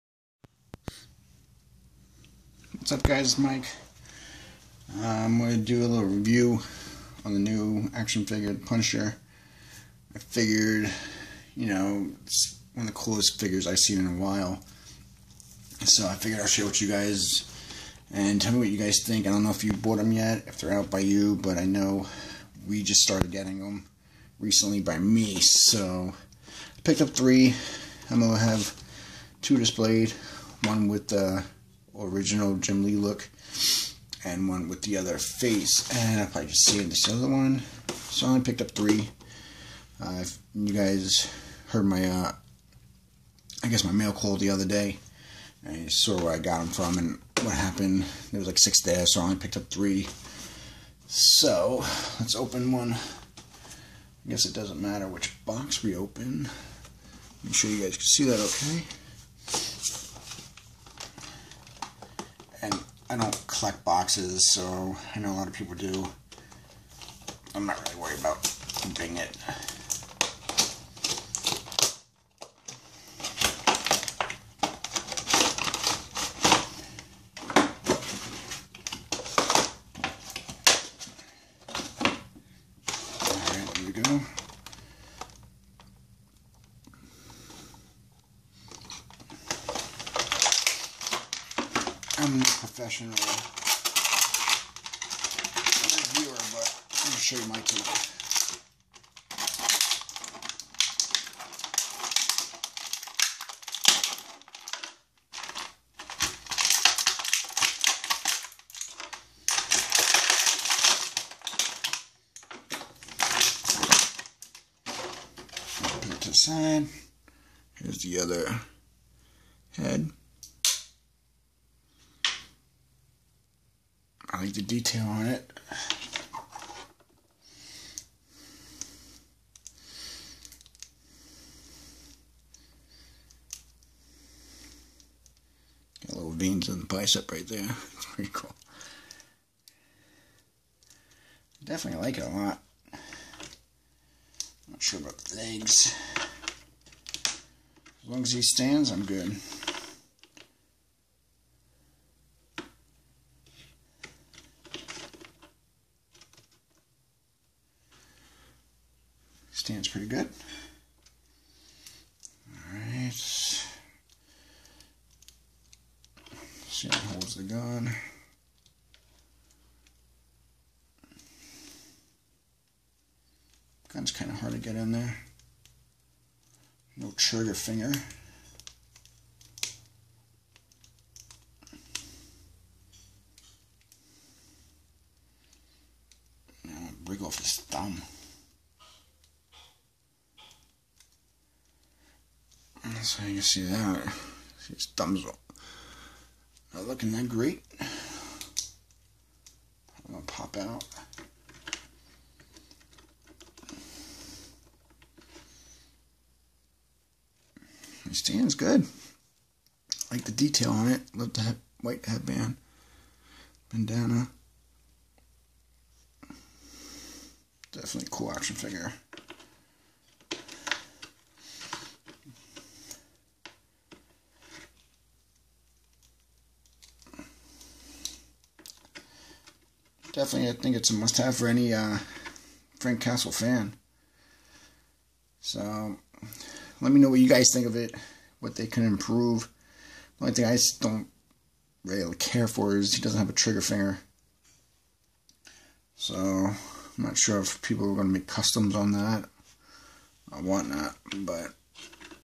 New Marvel Legends The Punisher Review!
Loving this guy, tell me what you guy's think? Definitely a must have for the Frank Castle fan.